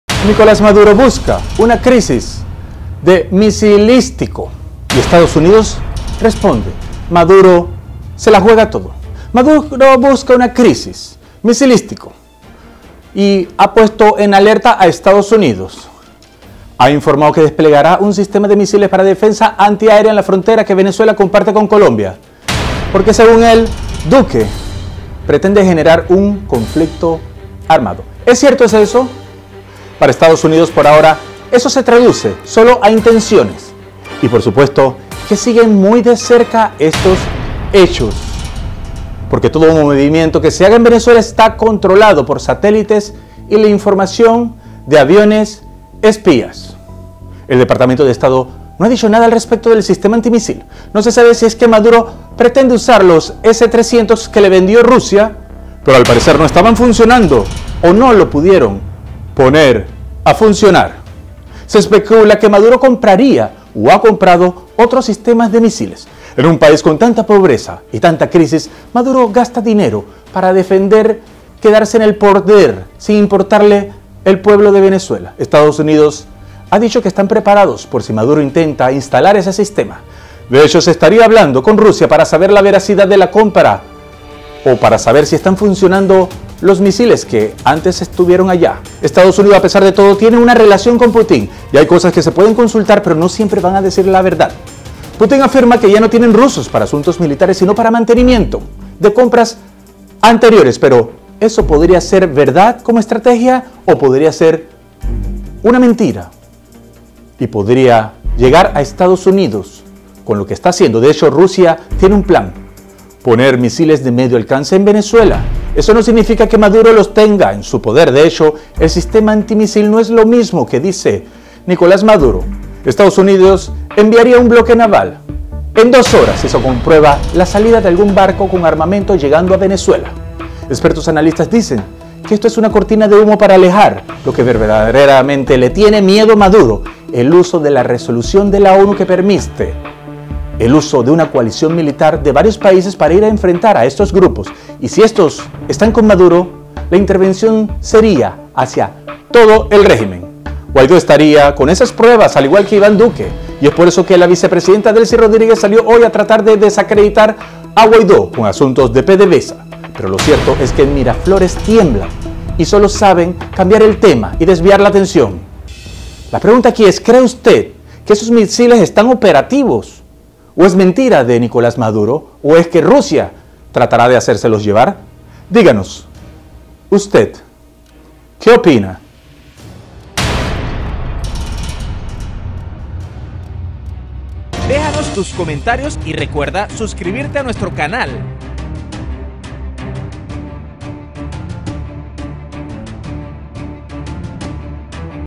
Maduro busca una crisis misilistico EEUU responde
Maduro busca crisis de misilístico EEUU responde. 5 septiembre 2019. Maduro se lo juega todo.

Este es un analisis de opinion sobre la situacion actual de Venezuela y su relacion con los EEUU y el mundo.
Fotos y o videos son para ilustra y enriquecer el contenido
Videos y fotos del Gobierno de Juan Guado by @Presidencia_VE Twitter public domain
Putin video y fotos by Kremli.ru